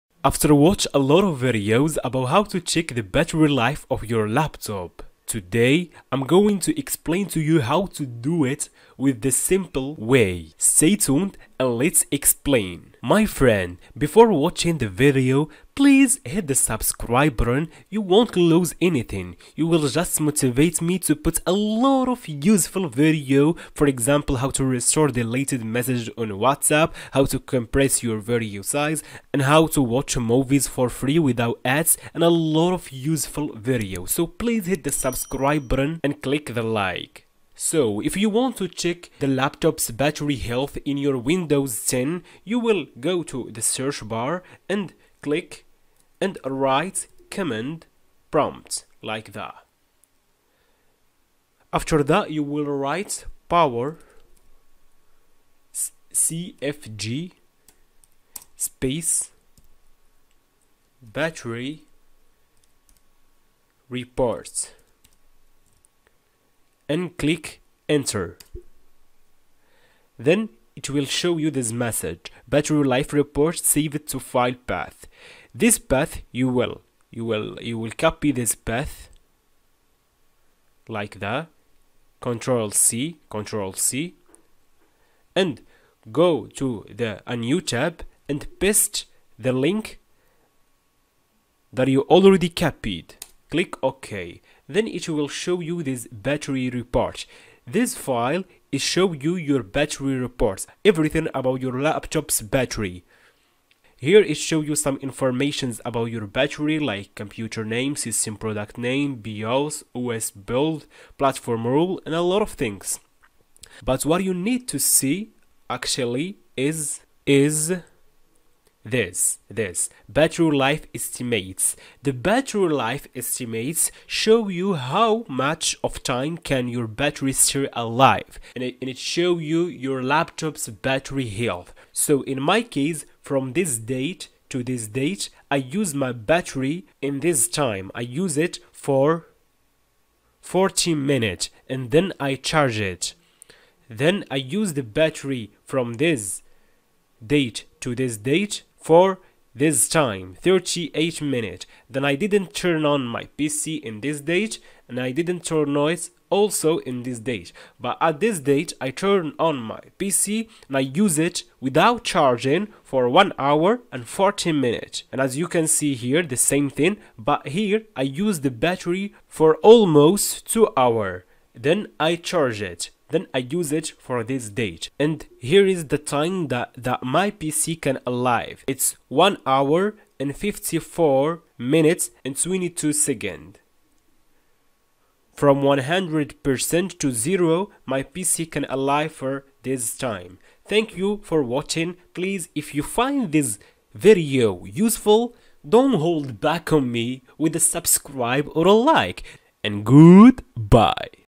How to check the battery life of your laptop in Windows 10
Today I'm going to teach you how to check the battery life of your laptop in Windows 10